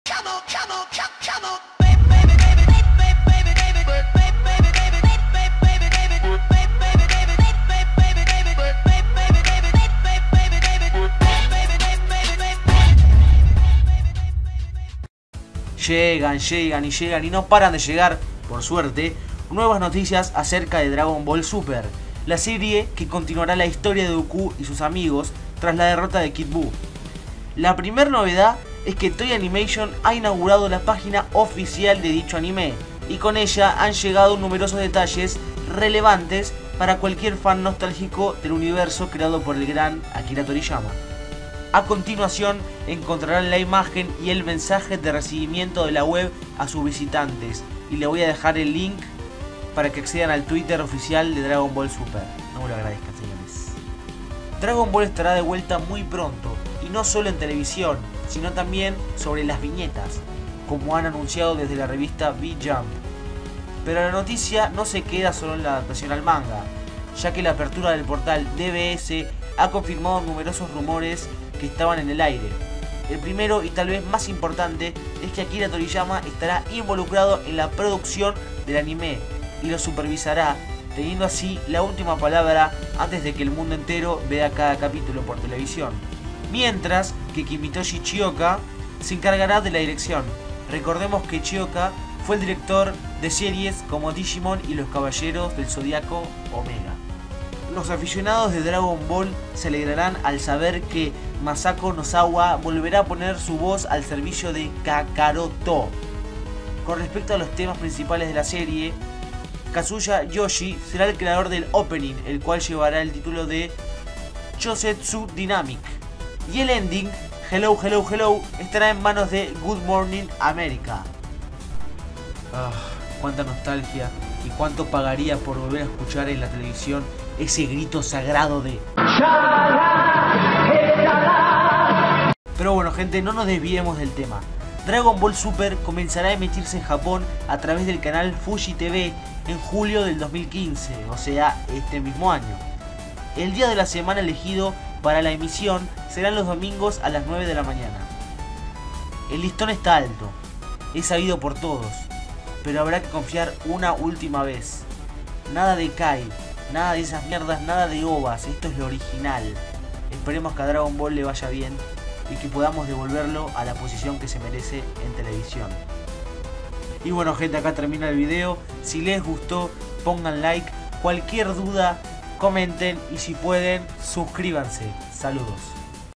DRAGON BALL SUPER | NUEVA INFORMACION
Hey que onda Gente de Youtube!
Aca su amigo ElSonyerl y hoy les traigo Informacion y sobre la nueva serie de Dragon ball Super!

►ID PSN: MARCOABYS
►Mi Skype: soymarquitoabys